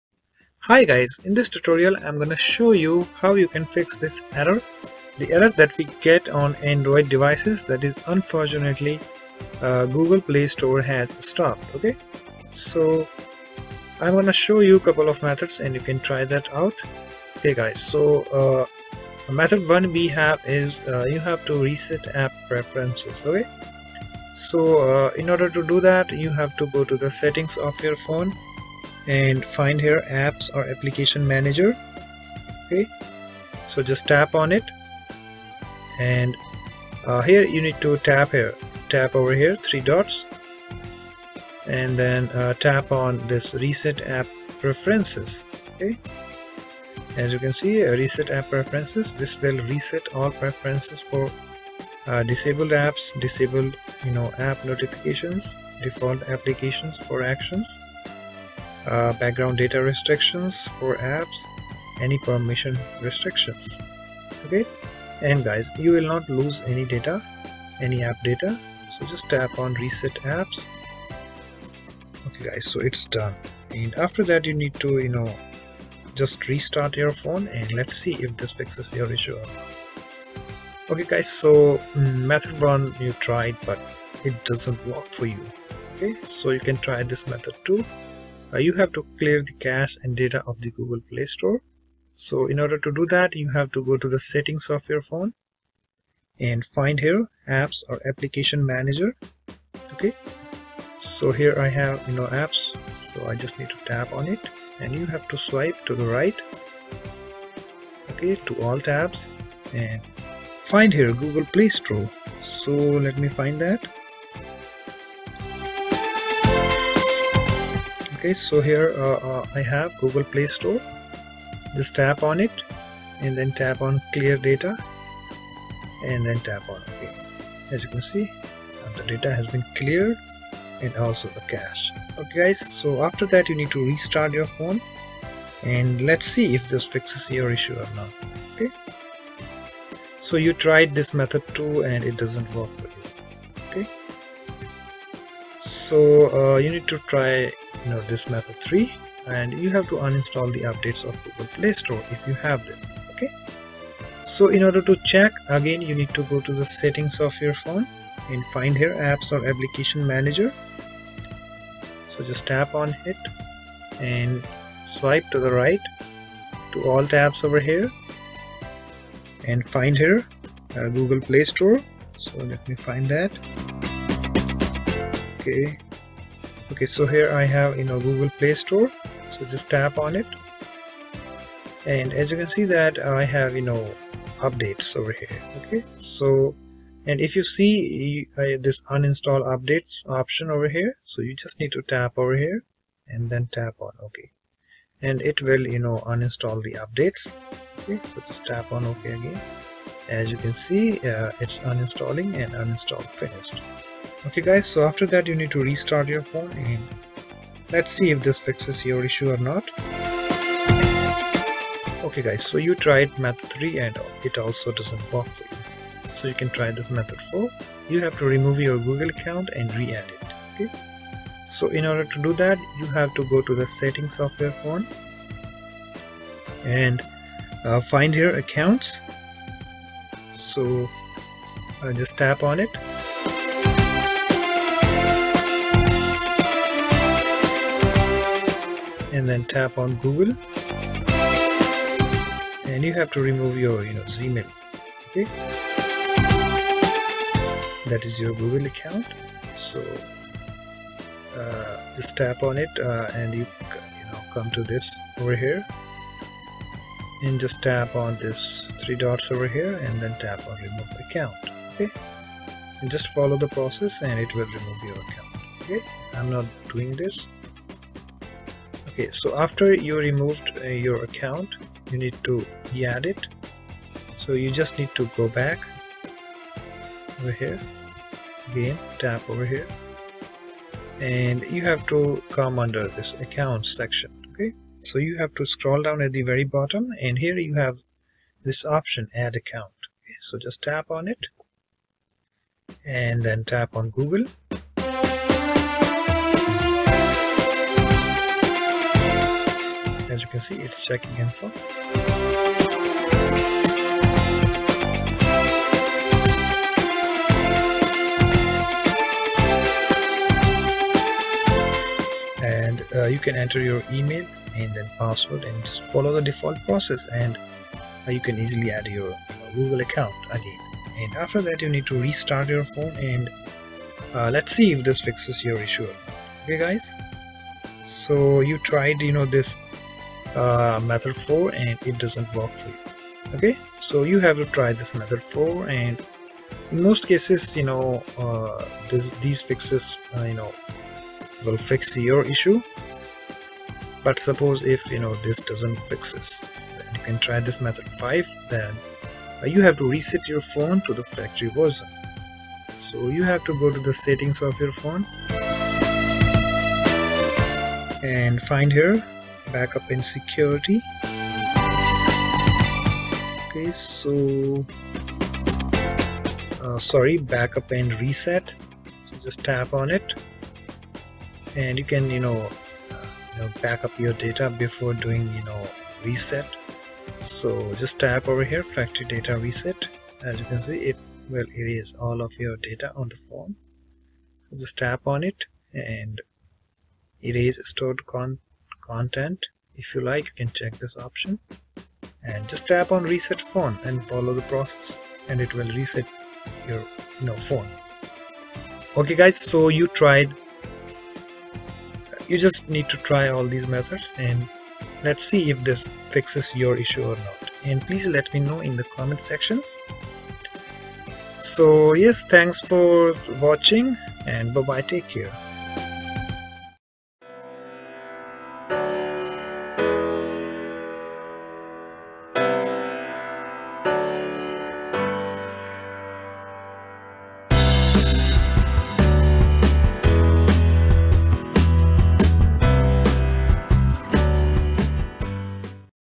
How To Fix "Unfortunately, Google Play store has stopped" Error on Android Devices ?
Method 1:

Reset App Preferences

Step 2: Tap on Apps or Application Manager.

Step 3: Tap on Menu ,and the Tap on Reset App Preferences .

Step 4: Tap on Reset Apps .

Step 5: Restart your Phone .

Method 2:

Clear the cache and data of the Google Play Store.

1. Go to Settings

2. Tap on Apps or Application Manager .

3. Select the "ALL" tab, Search for "Google Play Store" and tap to open it.

3. Tap "Force stop"

4. Tap "Clear cache"

5. Tap "Clear Data"

6. Now, Open Play Store.

Method 3:

Uninstall the Updates of Google Play Store

Step 3: Scroll to All Tabs .

Step 4: Find Google Play Store and Uninstall the Updates of it .

Method 4:

Remove Google Account and re-add it .

Step 2: Go to Accounts.

Step 3: Tap On Google.

Step 4: Tap on Gmail .

Step 5: Tap on Three Dots and Remove Account.

Step 5: Re-add it .

Method 5:

Reset your Phone to the Factory Version .

That's all